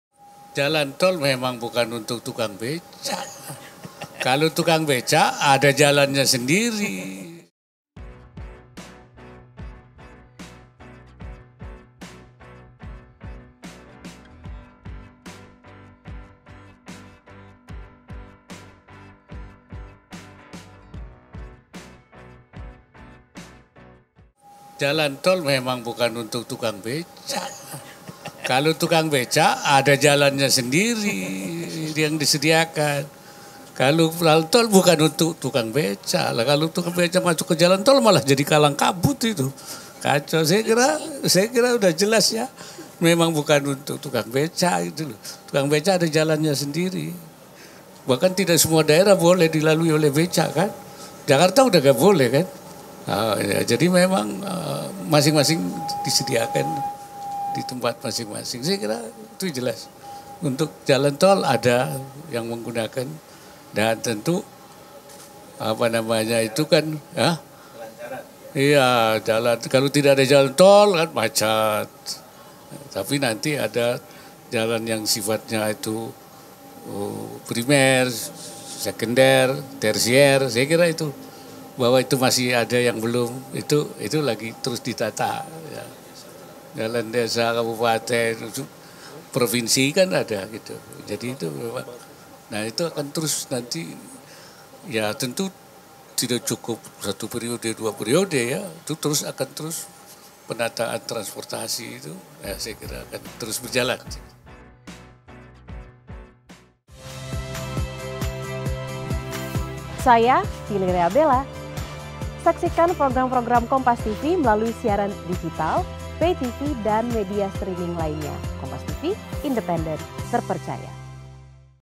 Momen Ma'ruf Amin Tanggapi Kritikan Cak Imin Perihal Manfaat Jalan Tol Tak Dirasakan Tukang Becak
JAKARTA, KOMPAS.TV - Wakil Presiden Maruf Amin tanggapi soal kritikan dari calon wakil presiden nomor urut 1, Muhaimin Iskandar yang menyebut soal manfaat tol yang tidak dirasakan oleh tukang becak.

Hal ini disampaikan Maruf Amin saat ditemui di Istana Wapres pada Rabu (20/12/2023)

Menurut Maruf Amin jika tukang becak masuk jalan tol malah akan menjadi kalang kabut.

"Kalau urusan jalan tol memang bukan untuk tukang becak, kalau tukang becak ada jalannya sendiri yang disediakan," ujar Maruf Amin.

"Kalau tukang becak masuk ke jalan tol, malah jadi kalang kabut," lanjutnya.